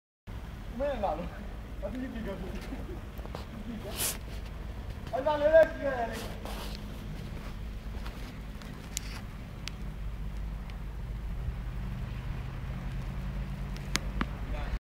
Neve C.Branco
Neve na cidade de Castelo Branco ( Noite de 10 de Janeiro na Rotunda Europa )